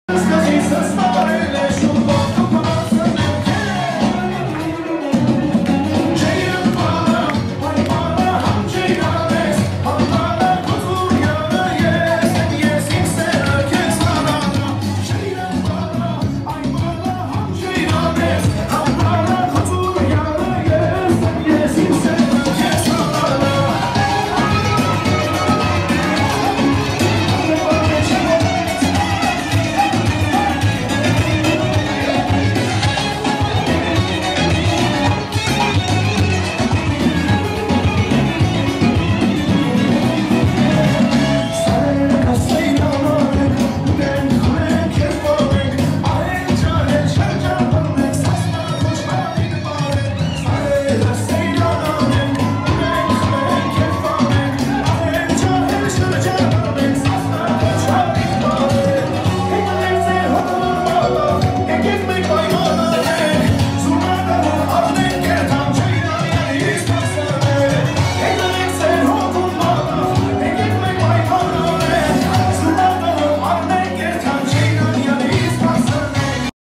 Armenian Urmia Society - Sheikhani
Armenian Urmia Society dinner dance 6-14-14 dancing Sheikhani.  Urmia is a city in Northwest Iran where a large population of Armenians lived / live.